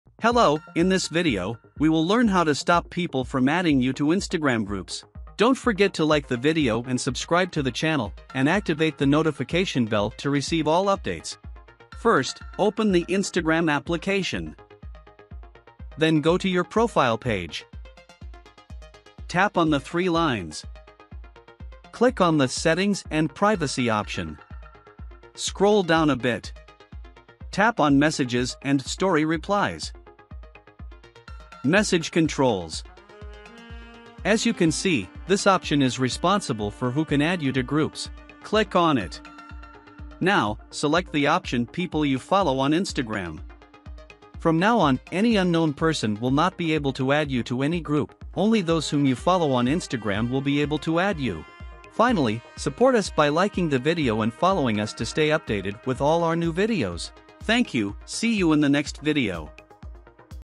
how to stop people from adding you to instagram groups
Are you tired of being constantly added to random groups on Instagram? In this video, we'll show you effective methods to stop people from adding you to Instagram groups without your consent. By following these steps, you can regain control over your group invitations and enjoy a more personalized Instagram experience.